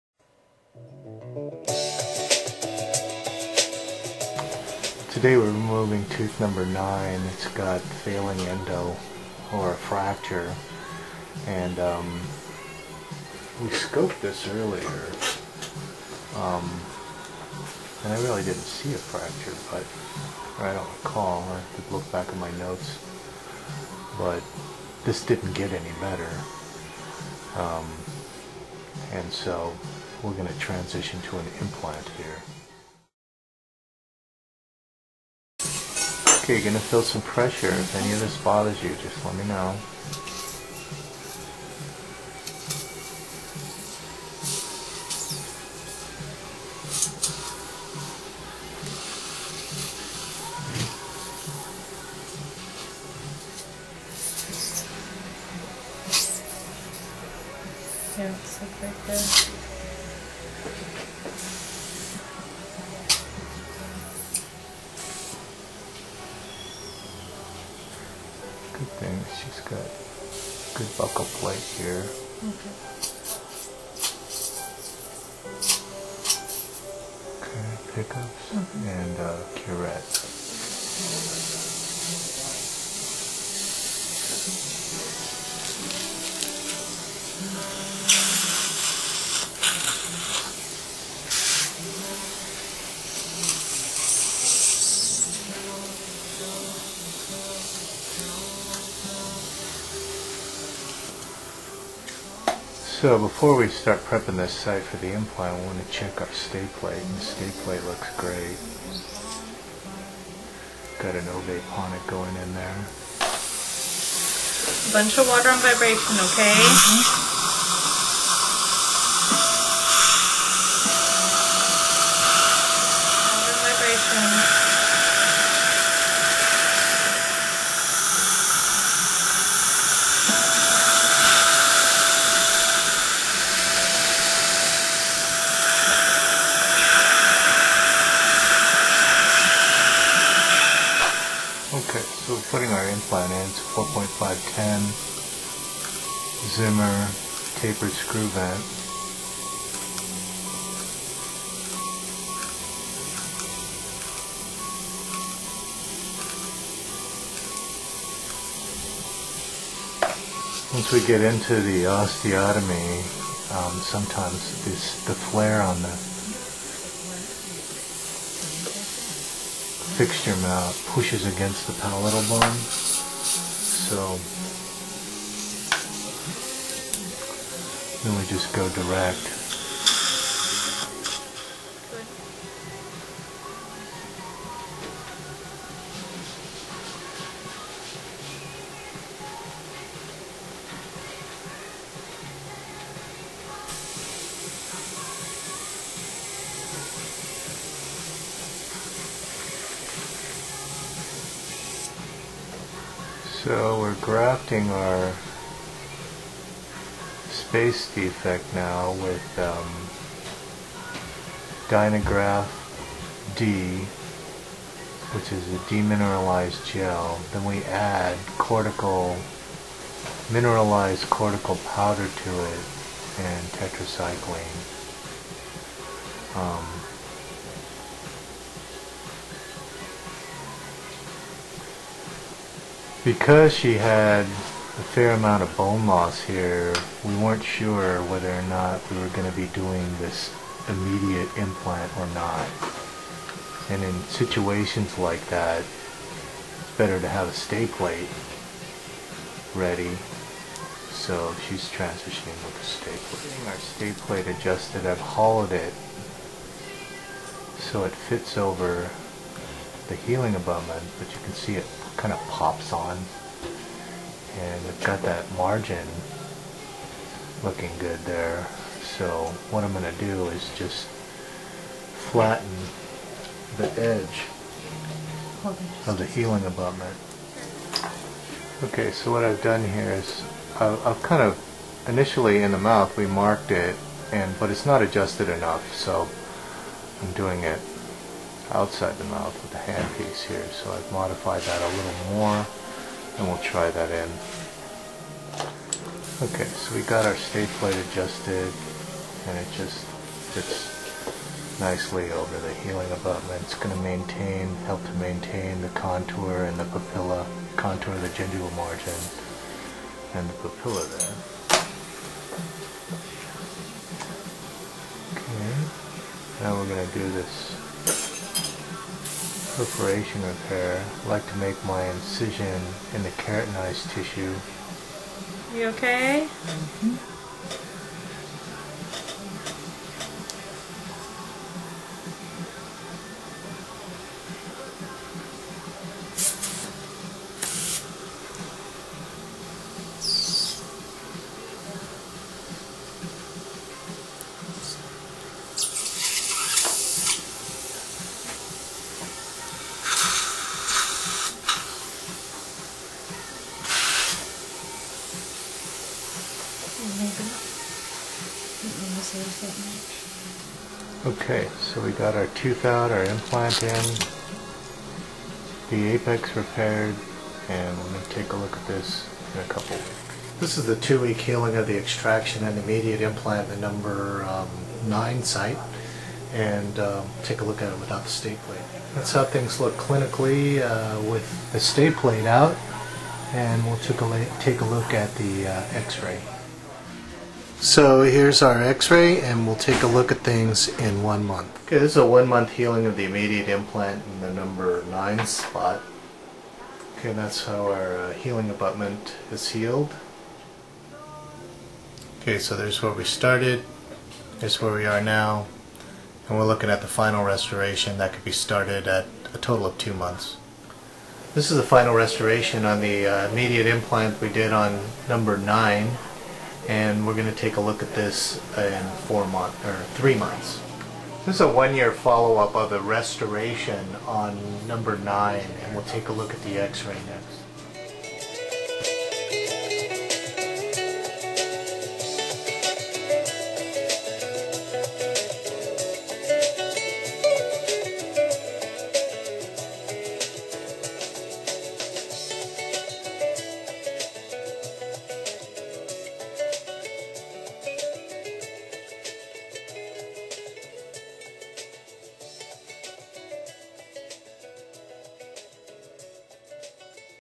Implant Microsurgery: Immediate Implant and Transition with Stayplate
Featuring: Global Microscope, Zimmer TSV Implant, Dynagraft D/FDB Bone Putty, Hartzell Microsurgery Instruments